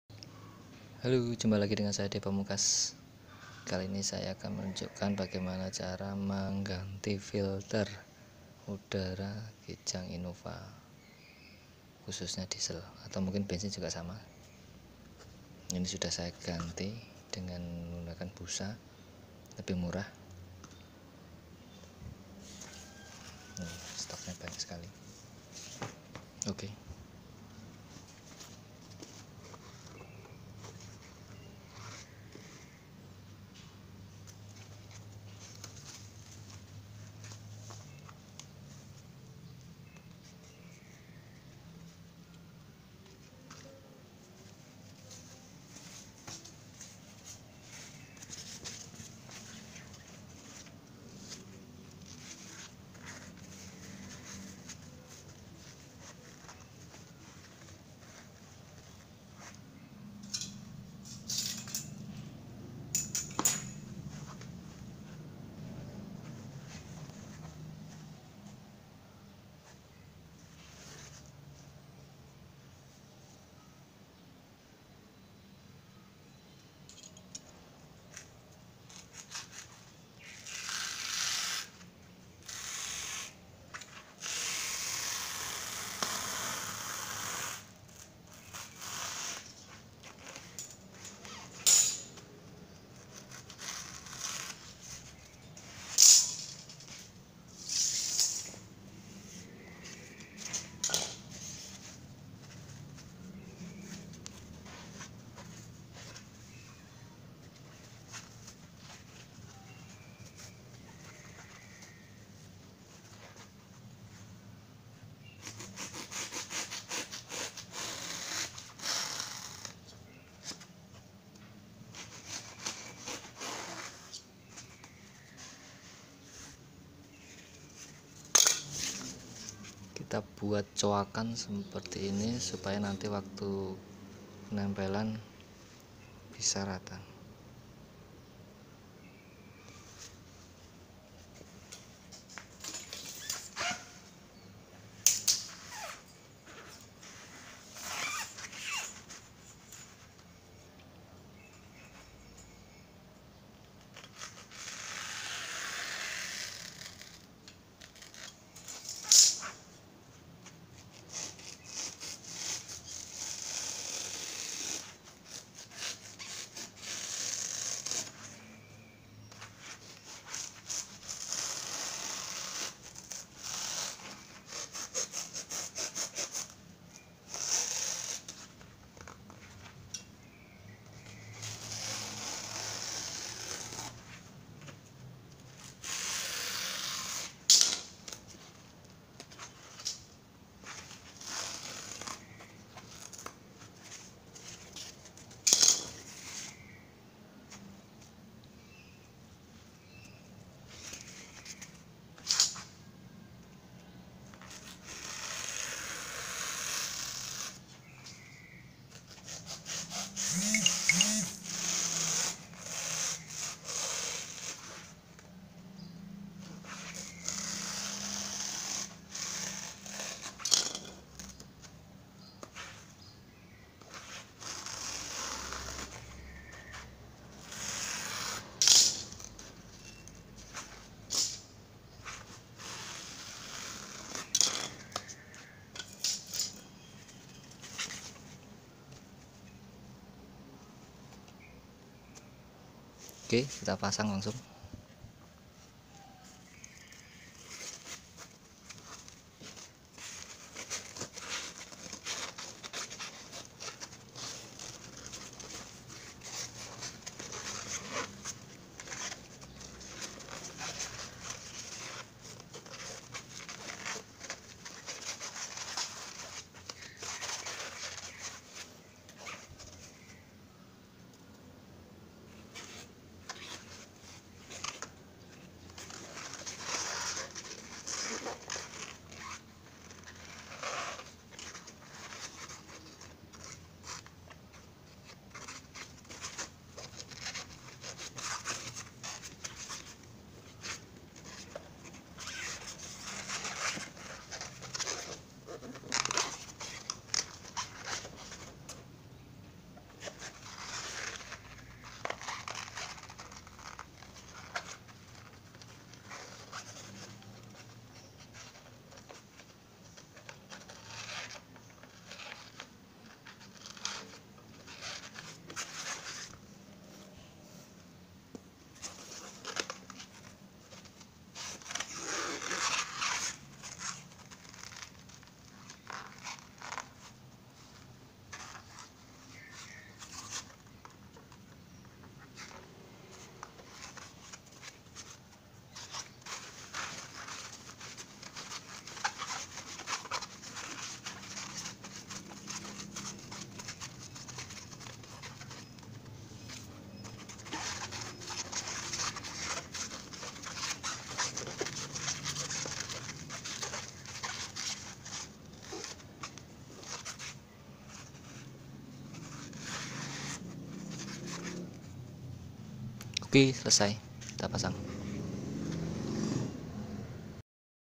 Modifikasi Filter Udara Kijang Innova
Berikut bagaimana cara mengganti filter udara kijang innova dengan menggunakan busa/spon yang semula berbahan kertas. Perbedaan sangat banyak..diantaranya isapan menjadi lebih enteng,efek penyaringan juga bagus karena kepadatan spon cukup tinggi (lubang pori kecil). Dengan biaya murah per meter hanya 20ribu, bisa digunakan hampir 20x penggantian..selamat mencoba..